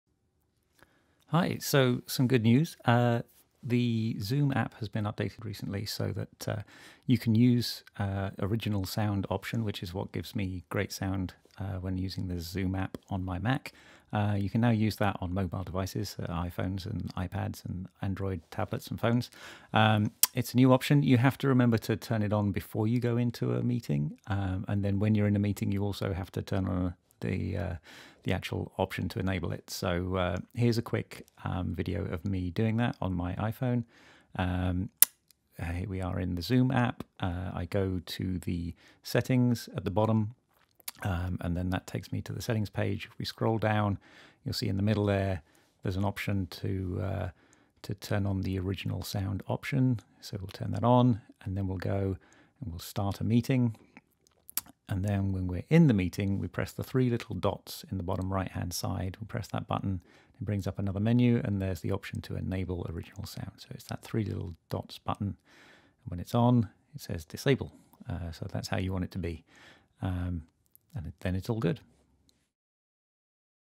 Getting Good Audio For Music In Zoom On Phones & Tablets
Previously I created a video explaining the benefits of enabling the "Original Sound" option in Zoom.  At the time that was limited to the PC and Mac versions of Zoom, but they've now enabled that option on phones and tablets too.  Here's a very quick video showing you how to turn it on.